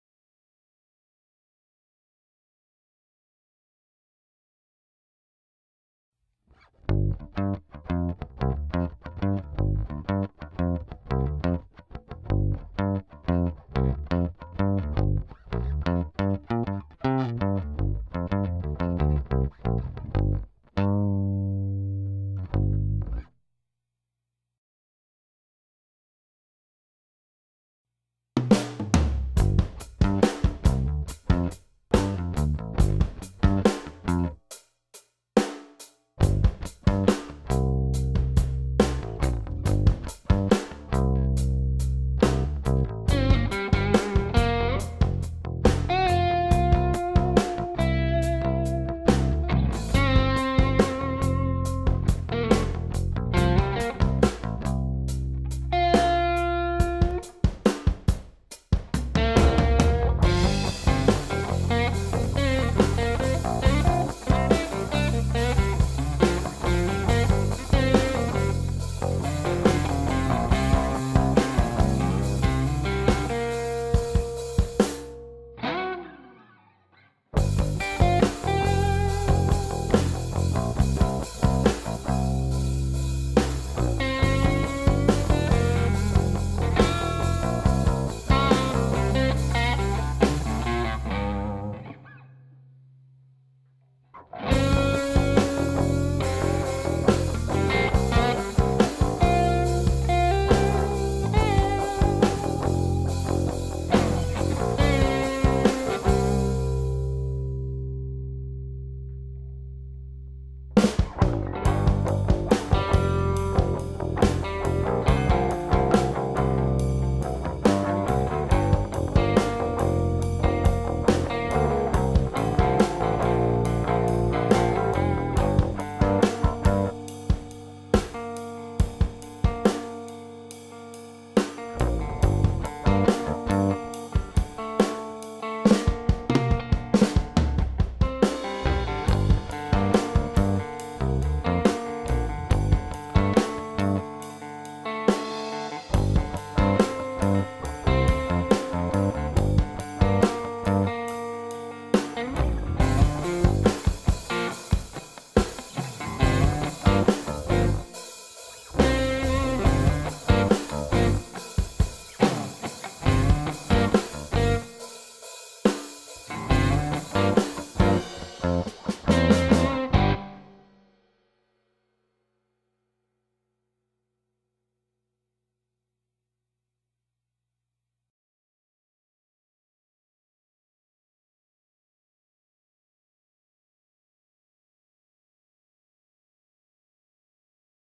Electro-Harmonix - Bass Soul Food - Pedal Demo Bass & Guitar, with Drums
EH's - Bass Soul Food Pedal - Boost/Overdrive (with Blend knob to mix dry and O/D signal..)  adds a bit of grit, tone and grind to your Bass...(for something more severe you will need to find a Bass fuzz) This pedal still benefits having an EQ after it to boost bottom end.